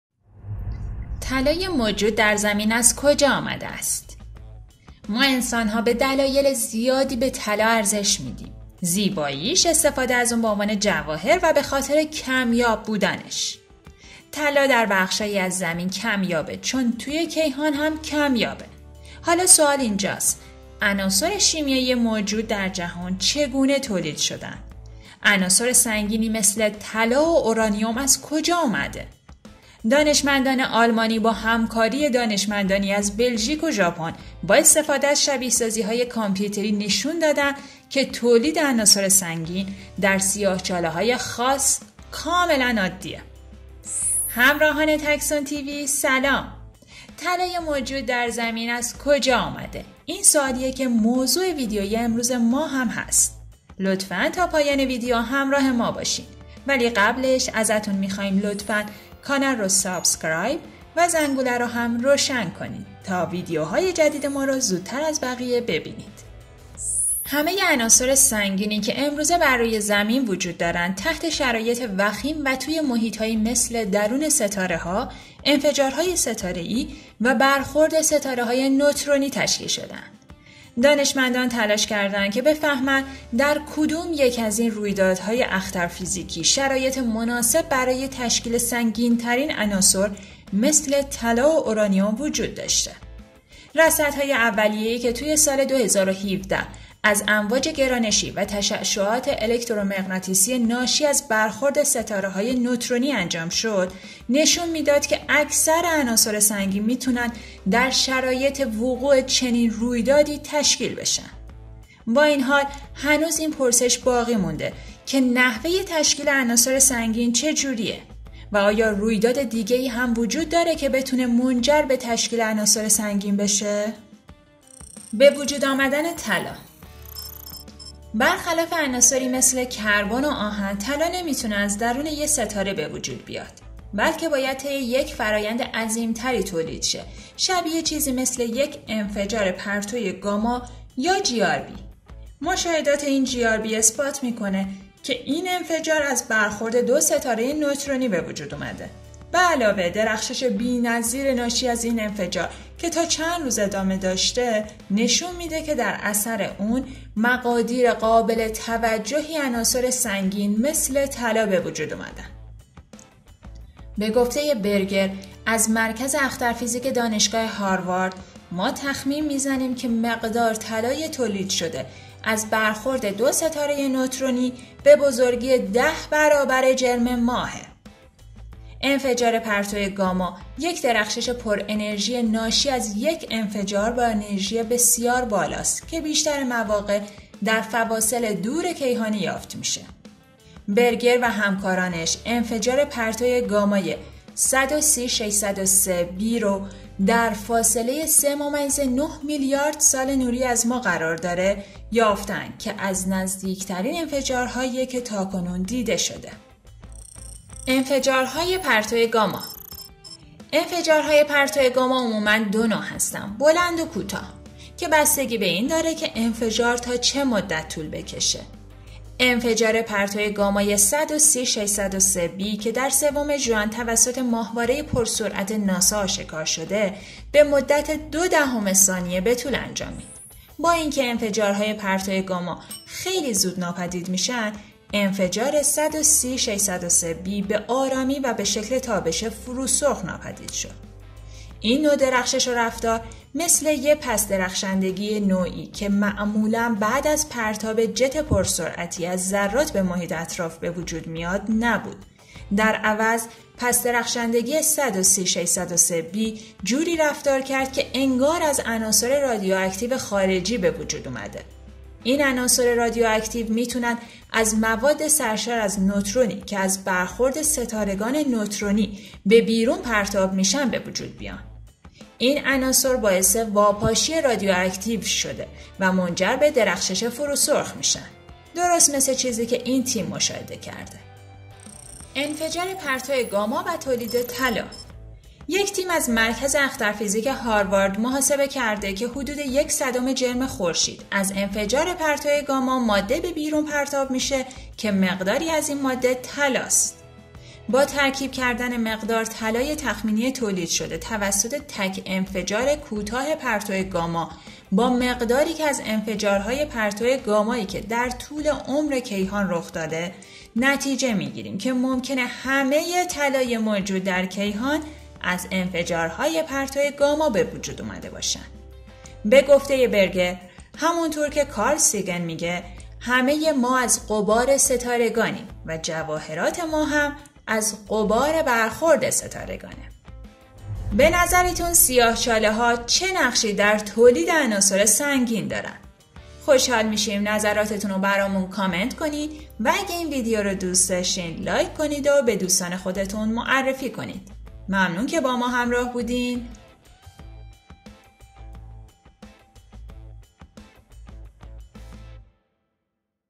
طلای موجود در زمین از کجا آمده است؟
سلام همراهان تکسون تی وی.خیلی خوشحالیم با یه ویدئو ی دیگه میبینمیتون
یادتون نره کانال رو سابسکرایب کنبد و با ما سفر هیجان انگیزتون به دنیای علم و تکنولوژی رو شروع کنید

ما انسان‌‌ها به دلایل زیادی به طلا ارزش می‌دهیم: زیباییش، استفاده از اون به عنوان جواهر و به خاطر کمیاب بودنش. طلا در بخش‌هایی از زمین کمیاب است چون در کیهان هم کمیابه.حالا سوال اینجاست،عناصر شیمیایی موجود در جهان چگونه تولید شده‌اند؟ و عناصر سنگینی مثل طلا و اورانیوم از کجا آمده‌اند؟ دانشمندان آلمانی با همکاری دانشمندانی از بلژیک و ژاپن با استفاده از شبیه‌سازی‌های کامپیوتری نشون داده‌اند که تولید عناصر سنگین در سیاه‌چاله‌های خاص کاملاً عادی است.

همه‌ی عناصر سنگینی که امروزه بر روی زمین وجود دارند، تحت شرایط وخیم و توی محیط‌هایی مثل درون ستاره‌ها، انفجارهای ستاره‌ای و برخورد ستاره‌های نوترونی تشکیل شده‌اند. دانشمندان تلاش کرده‌اند دریابند که در کدام یک از این رویدادهای اخترفیزیکی شرایط مناسب برای تشکیل سنگین‌ترین عناصر مثل طلا و اورانیوم وجود داشته. رصدهای اولیه‌ای که در سال ۲۰۱۷ از امواج گرانشی و تشعشعات الکترومغناطیسی ناشی از برخورد ستاره‌های نوترونی انجام شد، نشون می‌داد که اکثر عناصر سنگین می‌تونند در شرایط وقوع چنین رویدادی تشکیل شوند. با این حال، هنوز این پرسش باقی ماند که نحوه‌ی تشکیل عناصر سنگین چجوریه و آیا رویداد دیگه ای هم وجود دارد که بتونه منجر به تشکیل عناصر سنگین بشه؟
به وجود آمدن طلا
بر خلاف عناصری مثل کربن و آهن، طلا نمی‌تواند از درون یک ستاره به وجود بیاید. بلکه باید طی یک فرایند عظیم‌تری تولید شود- شبیه چیزی مثل یک انفجار پرتو گاما (GRB). مشاهدات این GRB اثبات می‌کند که این انفجار از برخورد دو ستاره‌ی نوترونی به وجود آمده. به علاوه درخشش بی‌نظیر ناشی از این انفجار که تا چند روز ادامه داشته، نشون می‌ده که در اثر اون مقادیر قابل توجهی عناصر سنگین مثل طلا به وجود آمده‌‌‌اند..به گفته‌ی برگر از مرکز اختر فیزیک دانشگاه هاروارد: «ما تخمین می‌زنیم که مقدار طلای تولید شده از برخورد دو ستاره‌ی نوترونی به بزرگی ۱۰ برابر جرم ماه است.» انفجار پرتو گاما، یک درخشش پر انرژی ناشی از یک انفجار با انرژی بسیار بالاست که بیشتر مواقع در فواصل دور کیهانی یافت می‌شود. برگر و همکارانش، انفجار پرتو گامای ۱۳۰۶۰۳B را که در فاصله‌ی ۳.۹ میلیارد سال نوری از ما قرار دارد، یافته‌اند که از نزدیک‌ترین انفجارهایی است که تا کنون دیده شده است.
انفجارهای پرتو گاما
انفجارهای پرتو گاما عموما دو نوع هستند: بلند و کوتاه- که بستگی به این داره که انفجار تا چه مدت طول بکشه. انفجار پرتو گامای ۱۳۰۶۰۳B که در سوم ژوئن توسط ماهواره‌ی پرسرعت ناسا آشکار شده به مدت دو دهم ثانیه به طول انجامید. با اینکه انفجارهای پرتو گاما خیلی زود ناپدید می‌شوند، انفجار ۱۳۰۶۰۳B به آرامی و به شکل تابش فروسرخ ناپدید شد. این نوع درخشش و رفتار، مثل یک «پس درخشندگی» نوعی که معمولا بعد از پرتاب جت پرسرعتی از ذرات به محیط اطراف به وجود می‌آید، نبود. در عوض پس درخشنگی ۱۳۰۶۰۳B جوری رفتار کرد که انگار از عناصر رادیواکتیو خارجی به وجود آمده. این عناصر رادیواکتیو می‌توانند از مواد سرشار از نوترونی که از برخورد ستارگان نوترونی به بیرون پرتاب می‌شوند به وجود بیایند. این عناصر باعث واپاشی رادیواکتیو گشته و منجر به درخشش فروسرخ می‌شوند. درست مثل چیزی که این تیم مشاهده کرده‌است.
انفجار پرتو گاما و تولید طلا
یک تیم از مرکز اختر فیزیک هاروارد محاسبه کردن که حدود یک صدم جرم خورشید، از انفجار پرتو گاما، ماده به بیرون پرتاب می‌شه که مقداری از این ماده طلا است. با ترکیب کردن مقدار طلای تخمینی تولید شده توسط تک انفجار کوتاه پرتو گاما با مقداری که از انفجارهای پرتو گامایی که در طول عمر کیهان رخ داده‌اند، نتیجه می‌گیریم که ممکن است همه‌ی طلای موجود در کیهان از انفجارهای پرتو گاما به وجود آمده باشد. به گفته‌ی برگر: «همانطور که کارل سیگن می‌گوید، همه‌ی ما از غبار ستارگانیم و جواهرات ما نیز ازغبار برخورد ستارگان است.»
به نظرتون، سیاه چاله ها چگونه بر تولید عناصر سنگین نقش دارند؟ خوشحال میشیم نظراتتون رو برامون کامنت کنید و اگه این ویدیو رو دوست داشتین لایک کنید و به دوستان خودتون معرفی کنید. ممنون که با ما همراه بودین